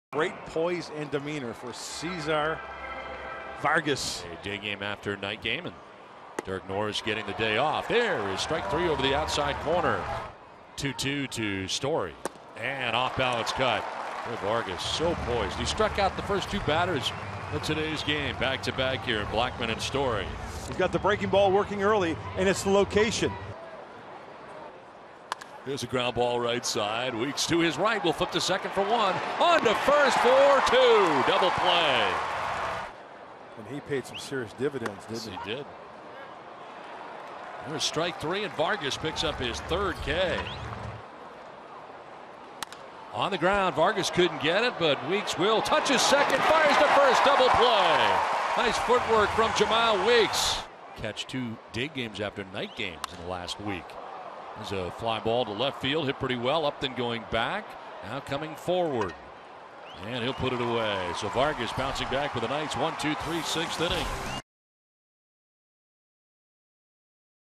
COL@SD: Vargas tosses six outstanding innings
Cesar Vargas throws six stellar innings, strikes out three, walks three and allows one run on four hits against the Rockies